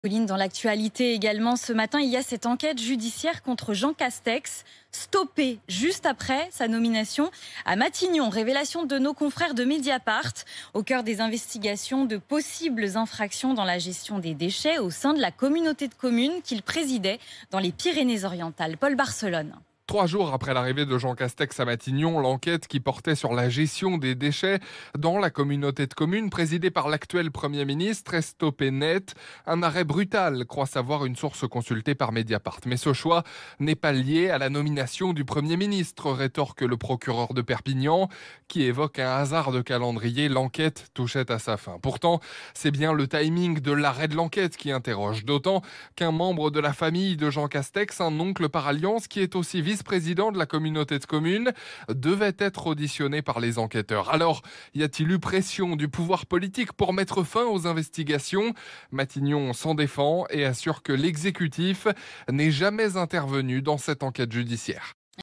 Une enquête judiciaire contre Jean Castex stoppée juste après sa nomination à Matignon
Révélations de Mediapart, il était soupçonné de possibles infractions dans la gestion des déchets au sein de la communauté de communes qu'il présidait dans les Pyrénées-Orientales